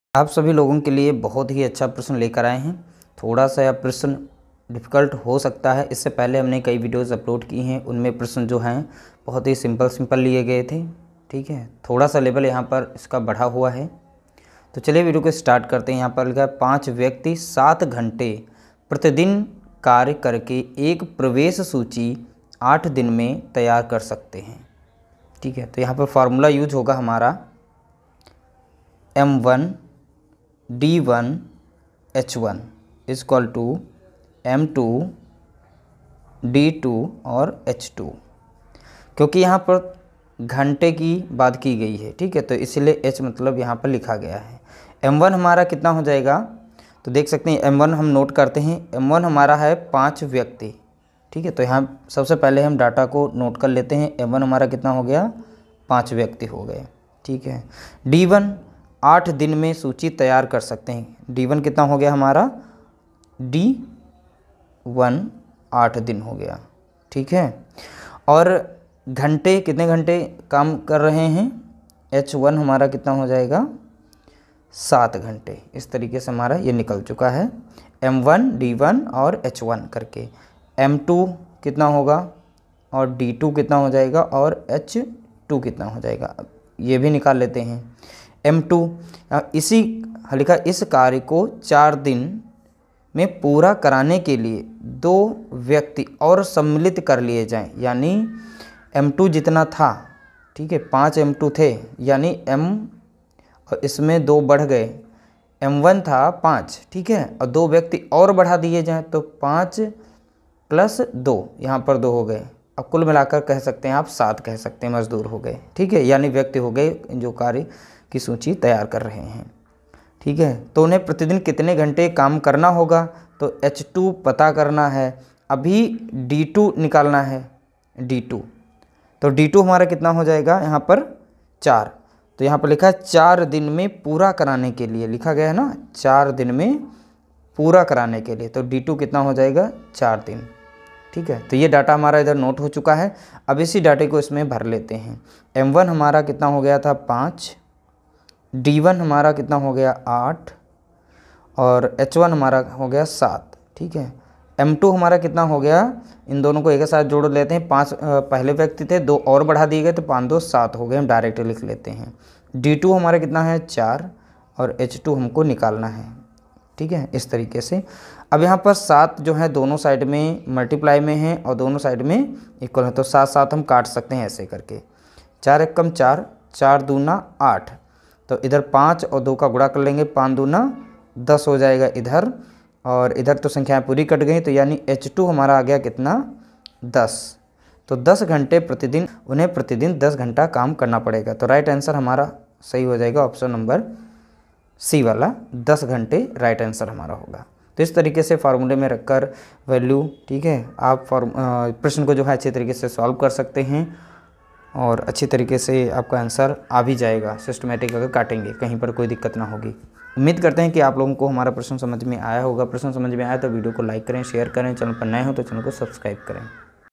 5 व्यक्ति 7 घंटे प्रतिदिन कार्य करके एक प्रवेश सूची 8 दिन में तैयार करते हैं,इस कार्य को 4 दिन में ?
5 व्यक्ति 7 घंटे प्रतिदिन कार्य करके एक प्रवेश सूची 8 दिन में तैयार करते हैं,इस कार्य को 4 दिन में पूरा कराने के लिए 2 व्यक्ति और सम्मिलित कर लिए जायें, तो उन्हें प्रतिदिन कितने घंटे काम करना होगा ?

5 persons working 7 hours a day prepare an admission list in 8 days, to complete this work in 4 days if 2 more persons are included, then how many hours per day will they have to work?